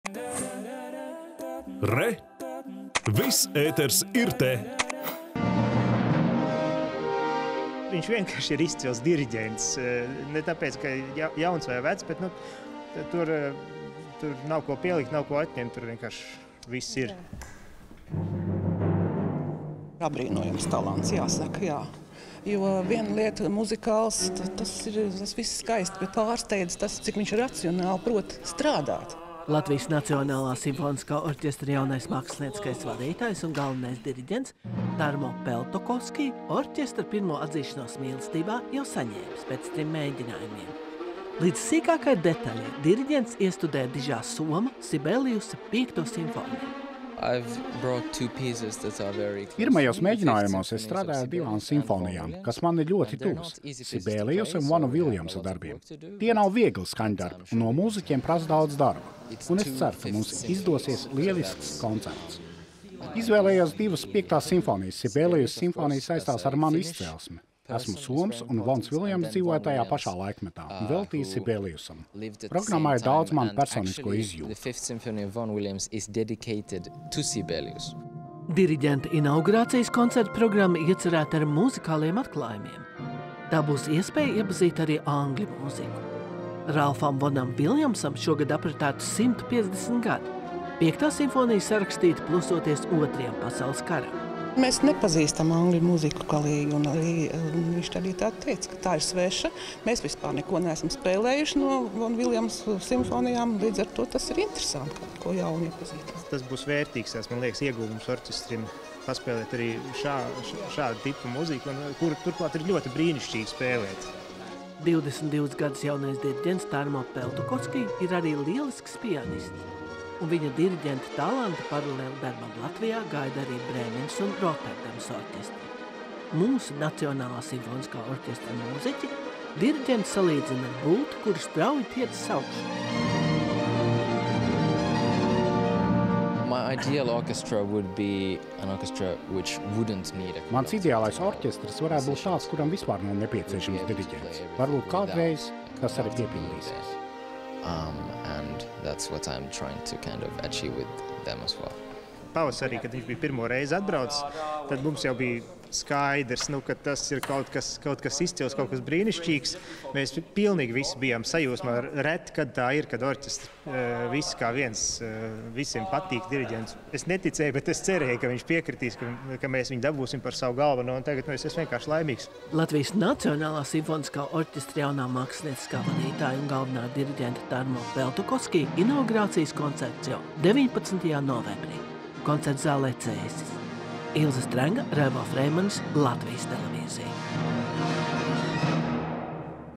LNSO jauns mākslinieciskais vadītājs - Tarmo Peltokoski
19. novembrī plkst. 17.00 koncertzālē “Cēsis” norisināsies Latvijas Nacionālā simfoniskā orķestra jaunā mākslinieciskā vadītāja un galvenā diriģenta Tarmo Peltokoski inaugurācijas koncerts.
  Īpašais notikums vērs jaunu lappusi Latvijas Nacionālā simfoniskā orķestra vēsturē, jo jau divas sezonas šis nozīmīgais amats gaidījis vislabāko pretendentu, kurš nu ir atrasts.
  Koncertā skanēs  Žana Sibēliusa, Ralfa Vona -Viljamsa un  Krista Auznieka skaņdarbi.
  Mēģinājumu klausījās žurnāliste Ilze Strenga.